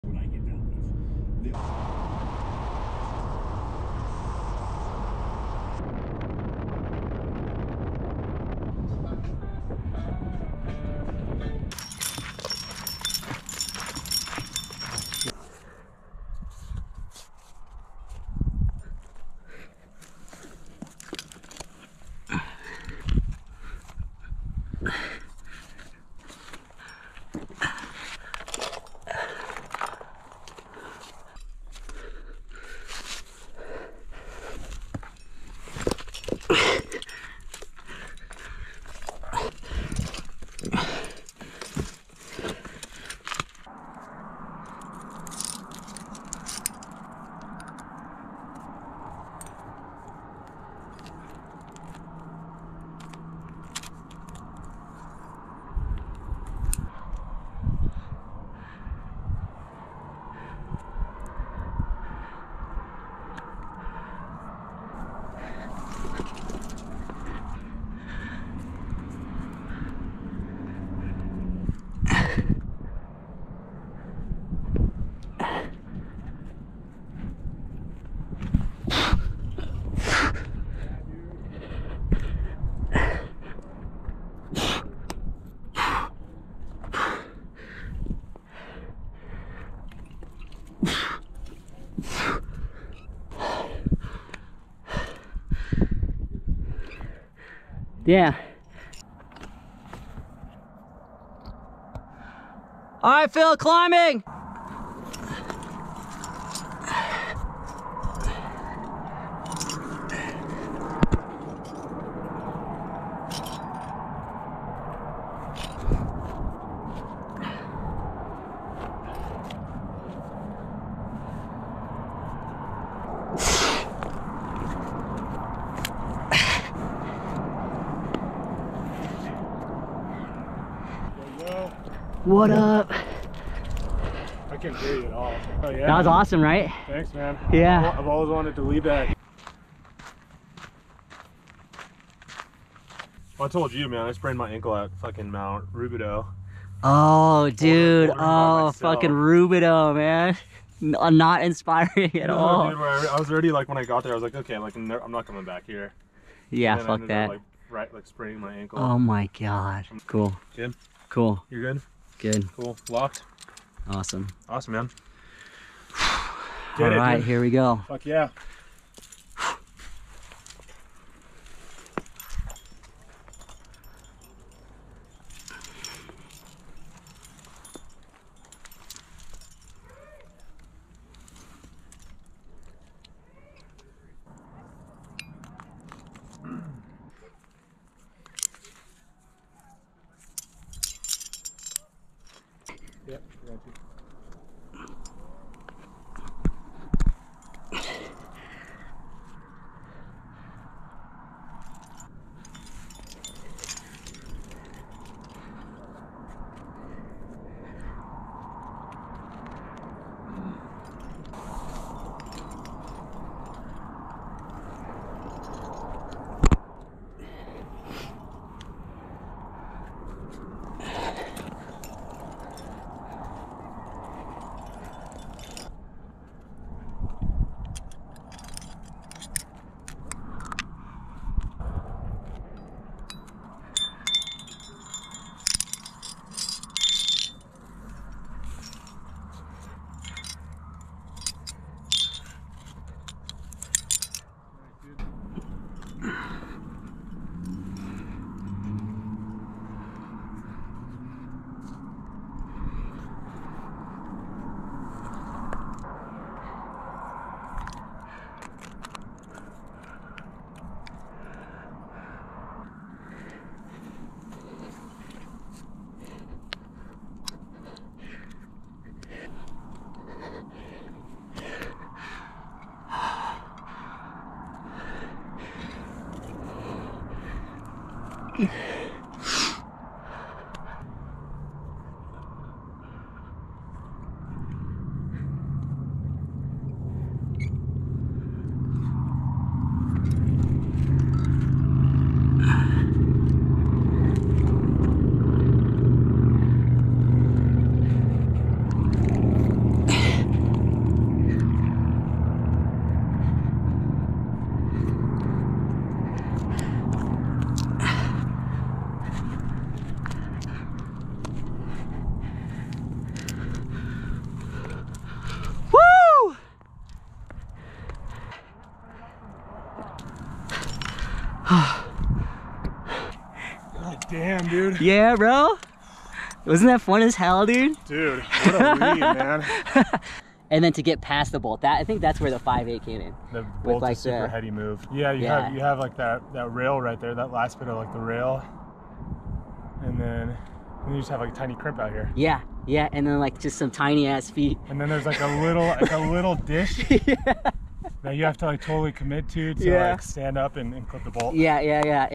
Joshua Tree - Overhang Bypass & Catch A Falling Star
Joshua Tree in January is a dream. I fractured my ankle after a fall last January so it felt extra nice to get out there. Clips from a day out climbing with Phil.

Footage from Overhang Bypass and Catch A Falling Star. Also did some repeats of the classics: Toe Jam, Sail Away and Mike's Books.